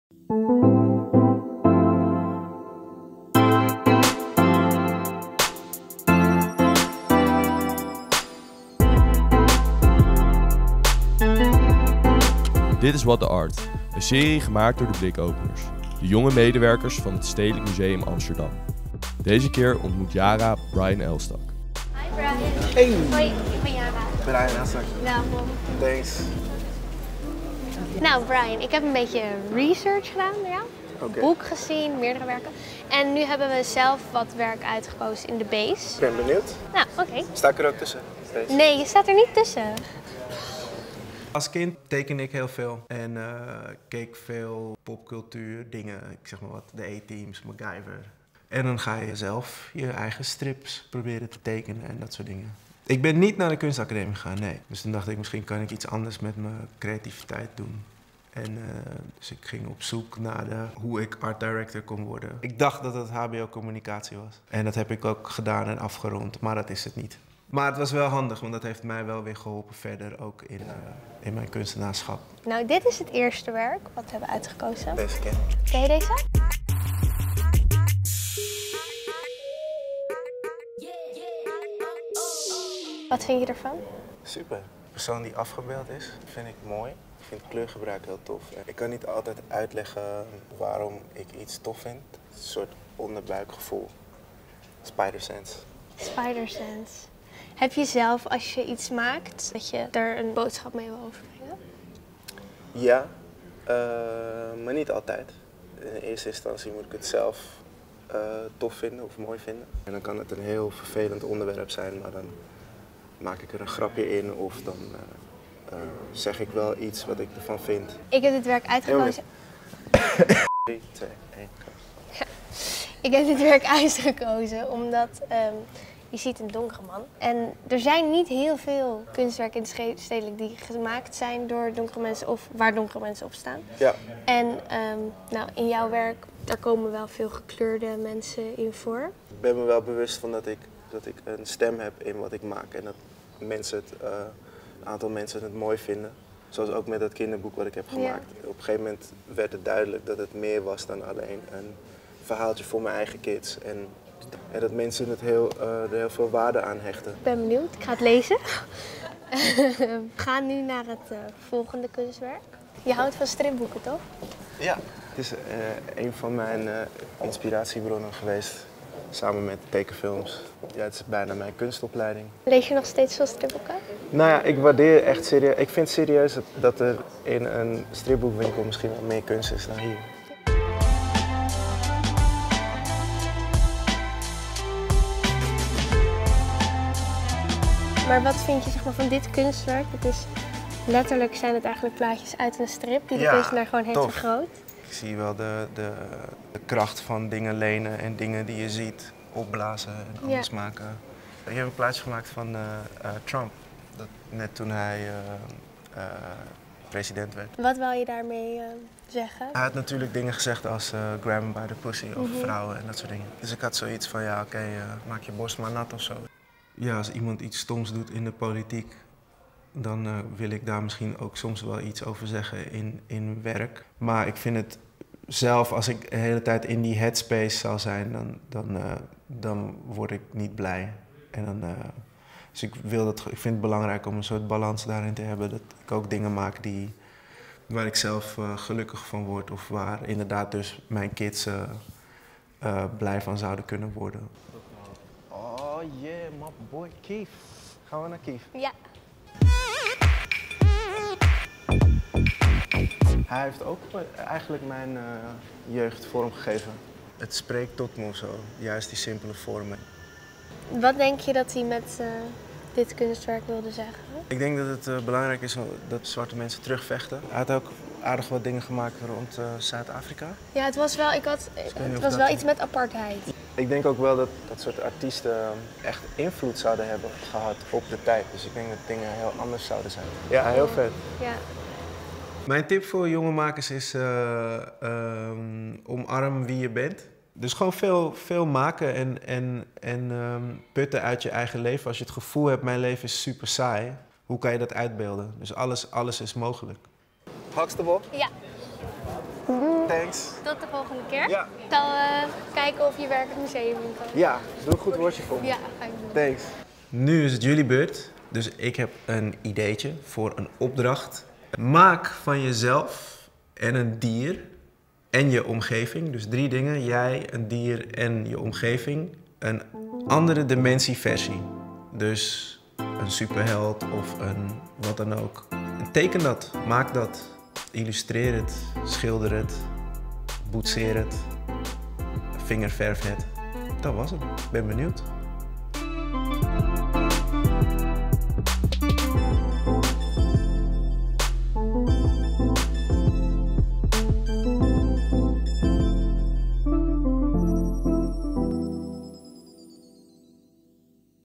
What The Art?! - Brian Elstak (Workshop)
What The Art?! is een serie gemaakt door de Blikopeners, de jongste medewerkers van het Stedelijk Museum Amsterdam. Deze keer gaat Yara op pad met illustrator Brian Elstak.